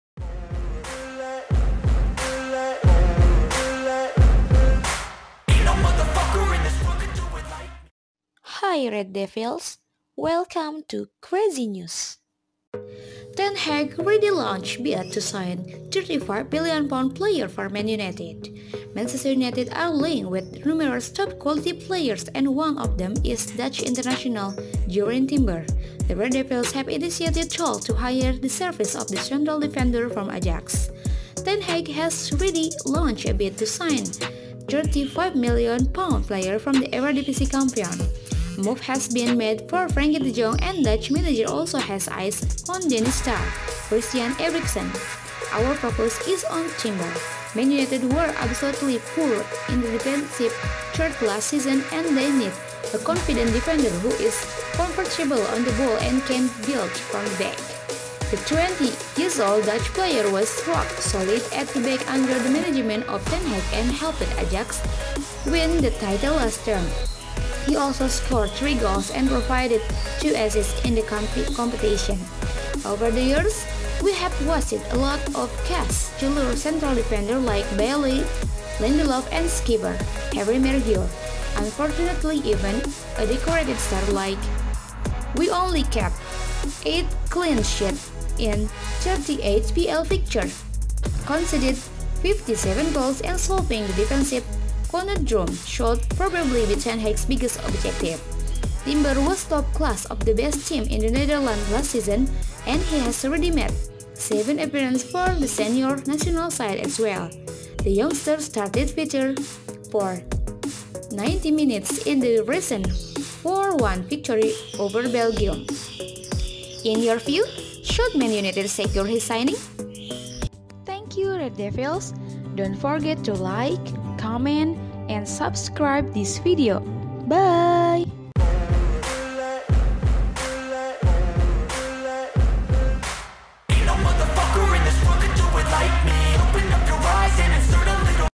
REPORT | Ten Hag has already launched bid to sign £35million player for Man United
Manchester United are linked with numerous top-quality players and one of them is Dutch international, Jurrien Timber.
the Red Devils have initiated talks to hire the services of the central defender from Ajax.